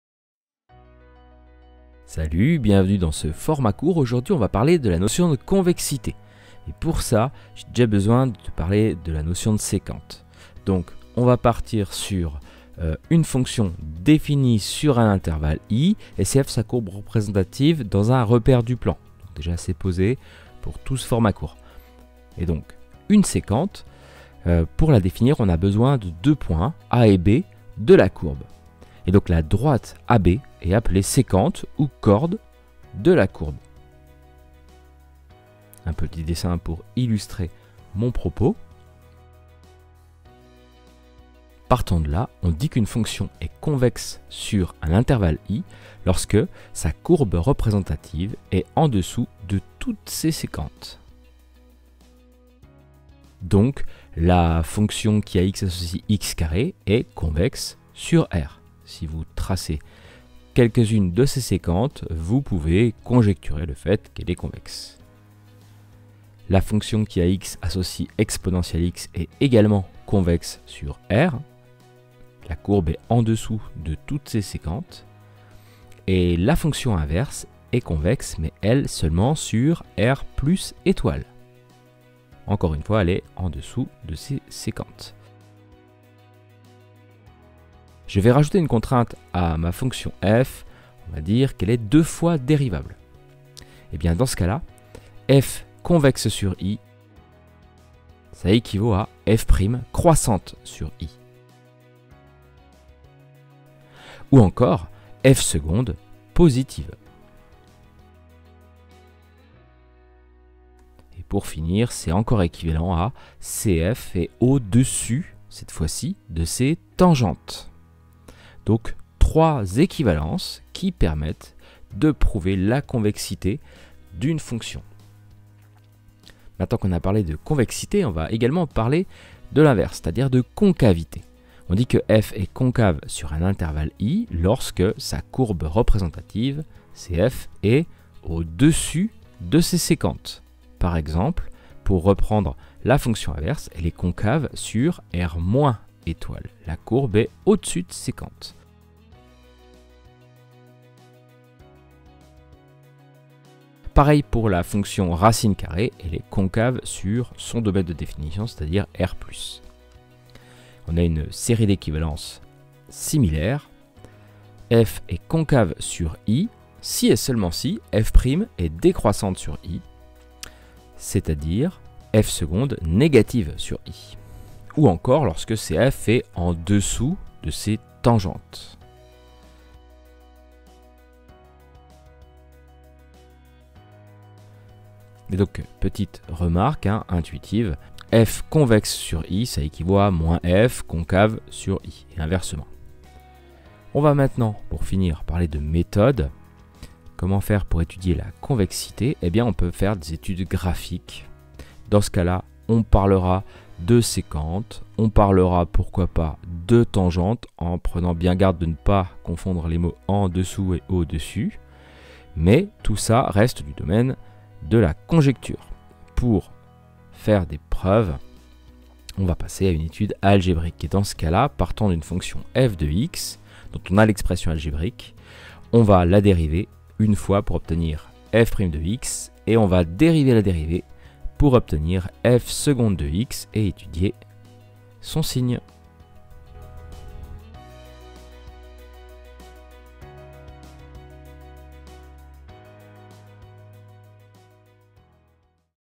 FC Term - Convexité
On définit ici la notion de convexité et concavité d'une fonction, deux fois dérivable sur un intervalle I.